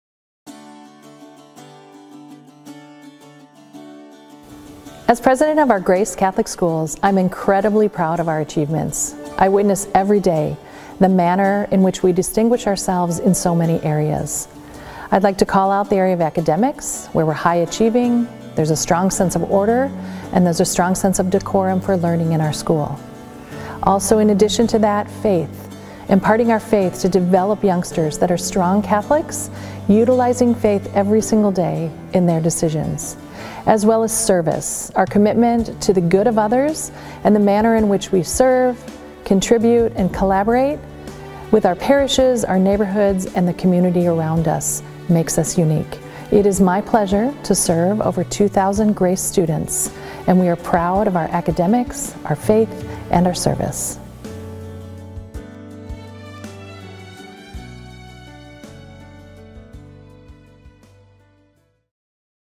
GRACE President Kimberly Desotell
Kimberly Desotell has served as President of the GRACE school system since 2015. Her leadership has been instrumental in helping grow enrollment while maintaining our schools' high academic standards and spiritual focus.